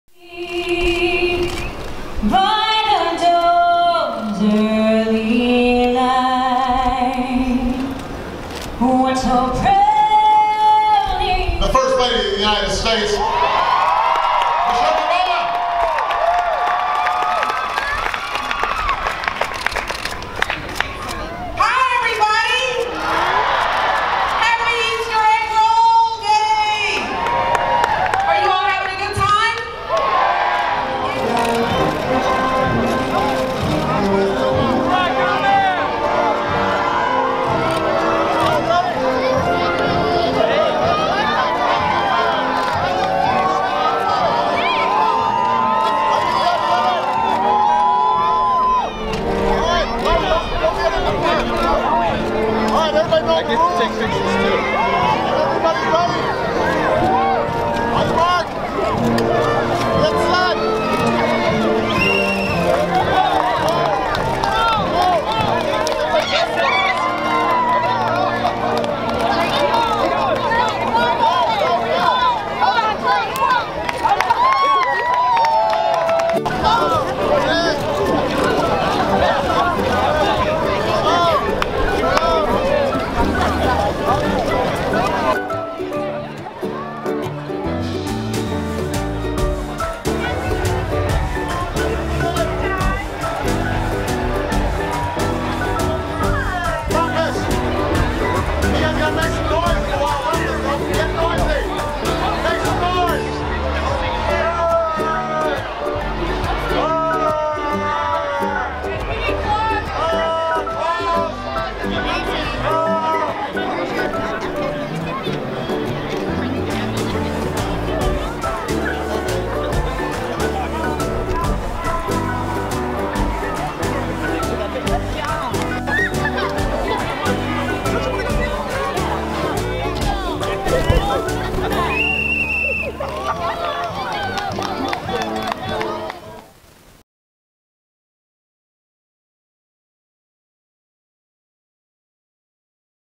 White House Easter Egg Roll 2016
More than 35,000 people visited the White House for the 138th annual White House Easter Egg Roll on Monday morning. Special guests included Idinza Menzel, Shaquille O'Neal, and Beyonce. This was the Obama family's last Easter Egg Roll.

Video by Deepa Shivaram